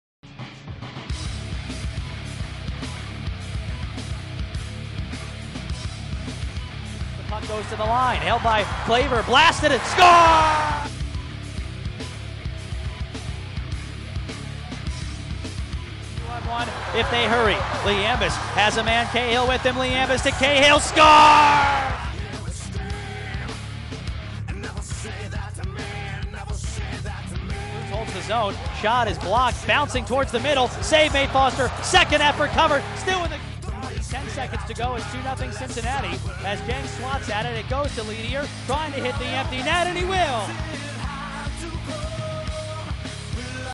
Cyclones vs Express - December 17, 2011
Cyclones beat the Express 3-0 behind a Brian Foster shutout at U.S. Bank Arena